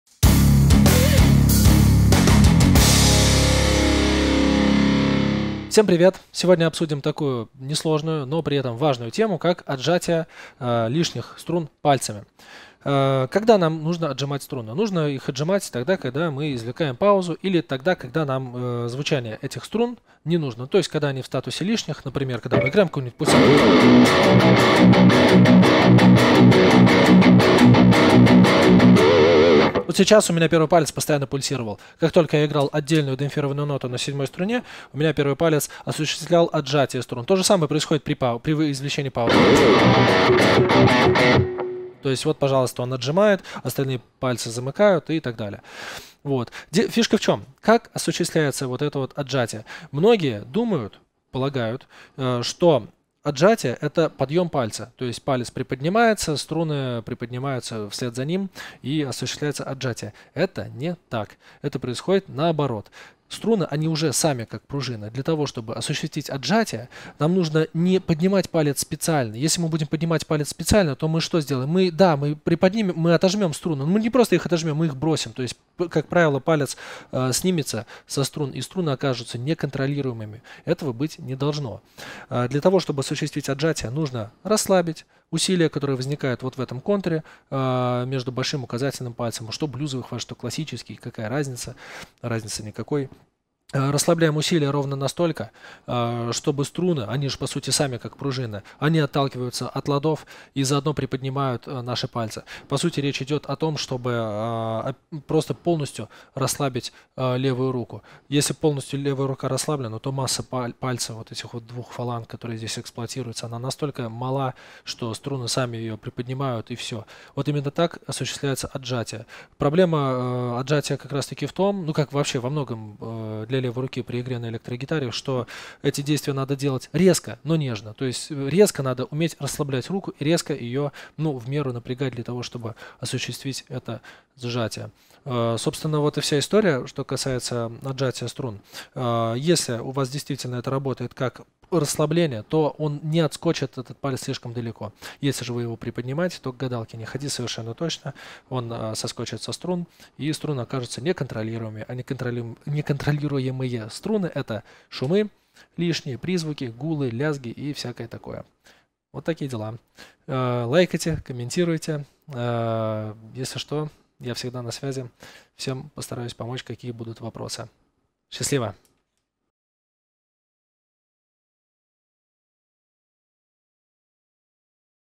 Как отжимать лишние струны
ЦИКЛ ВИДЕОУРОКОВ: РАСПРОСТРАНЕННЫЕ ОШИБКИ ПРИ ИГРЕ НА ЭЛЕКТРОГИТАРЕ
Как правильно отжимать лишние струны?

Этот видеоурок об одном небольшом, но очень важном аспекте, который отвечает за такие вещи, как паузы, чирки и чистоту.